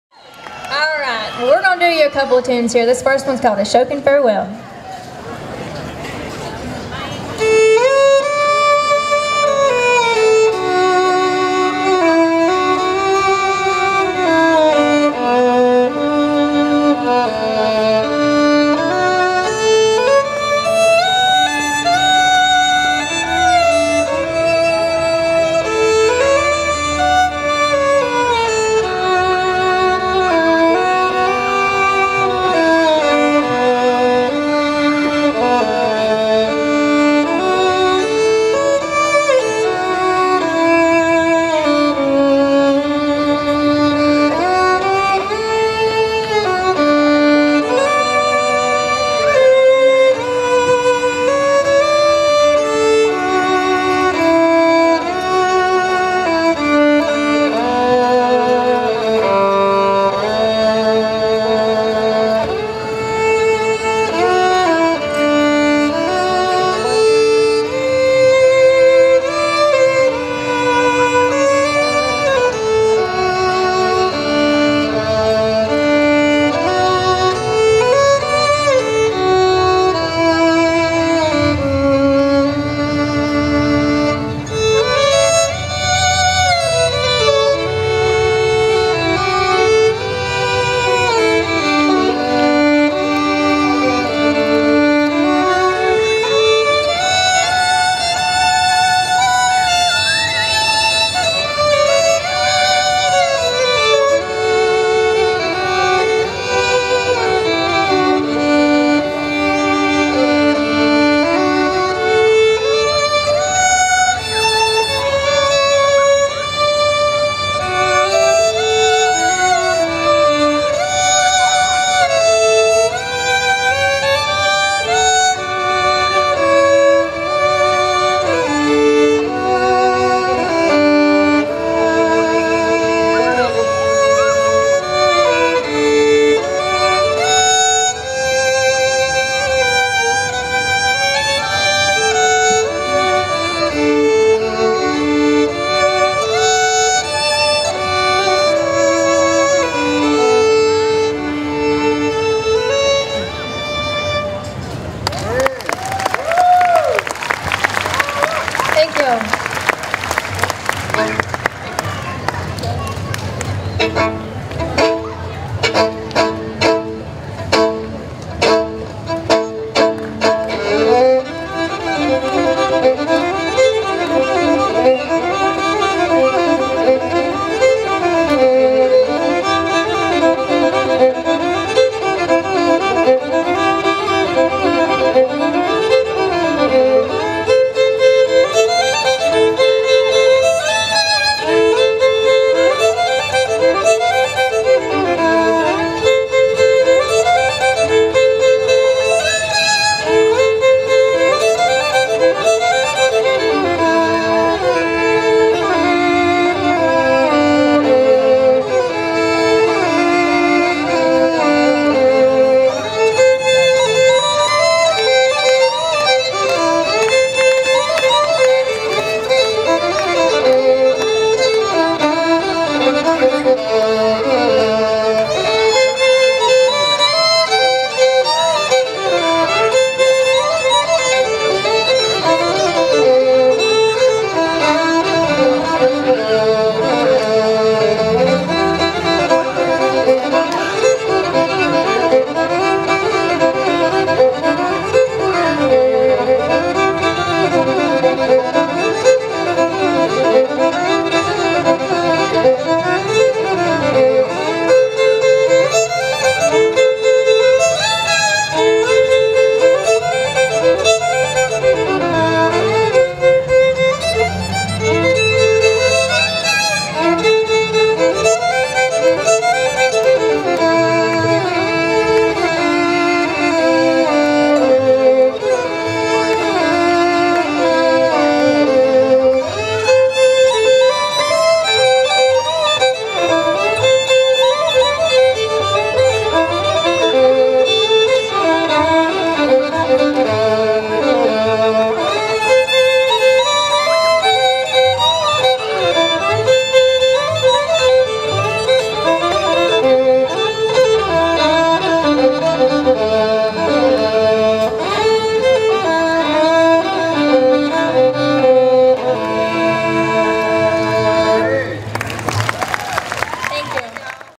Lillie & Carla at Shindig on the Green
Performed at the June 28, 2014 Shindig on the Green in Asheville, NC